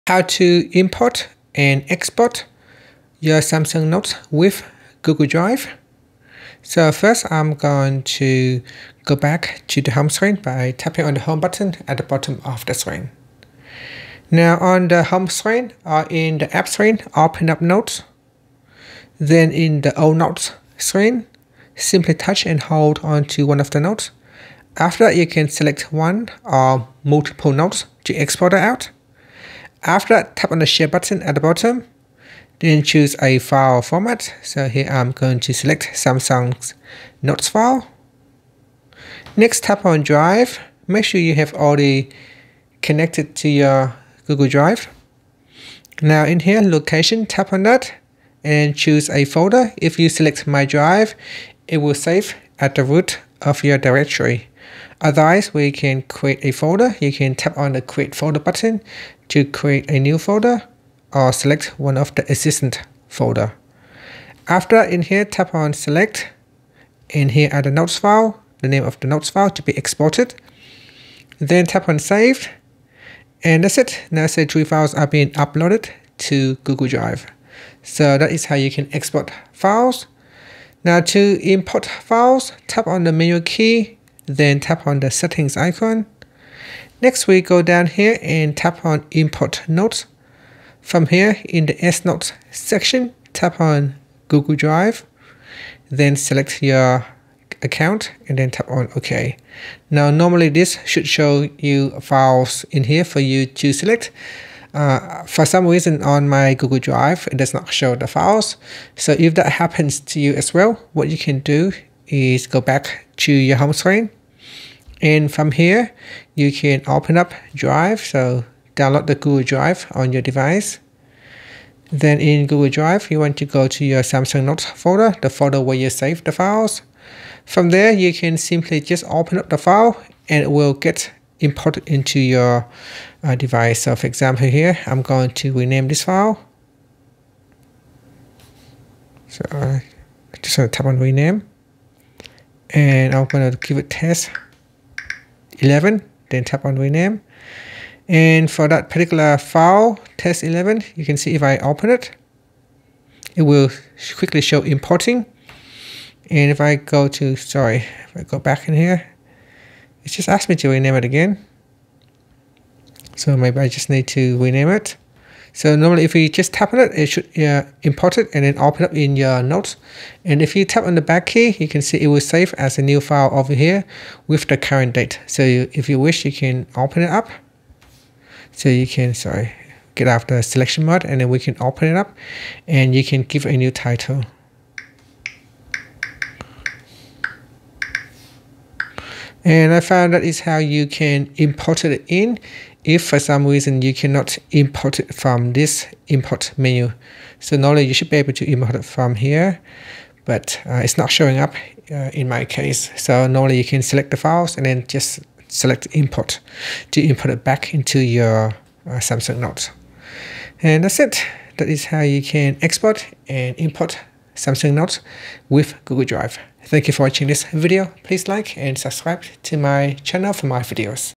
Samsung Notes x Google Drive: Effortless Backup & Import/Export
See how you can Import/Export Samsung Notes With Google Drive on Samsung phones.

Learn how to easily back up your Samsung Notes to Google Drive and then import them onto other devices. We'll cover exporting, importing, and troubleshooting for seamless note management.